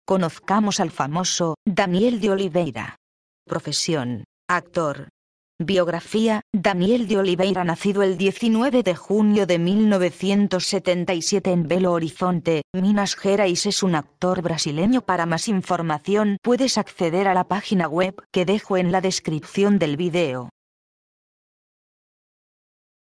Daniel de Oliveira - Actor
Conoce mas sobre el famosoDaniel de Oliveira Datos Generales: Daniel de Oliveira - Actor - Brasil || Pequeña Biografia de Daniel de Oliveira, Daniel de Oliveira nacido el 19 de junio de 1977 en Belo Horizonte, Minas Gerais es un actor brasileño Para mas información puedes acceder a la pagina siguiente de Oliveira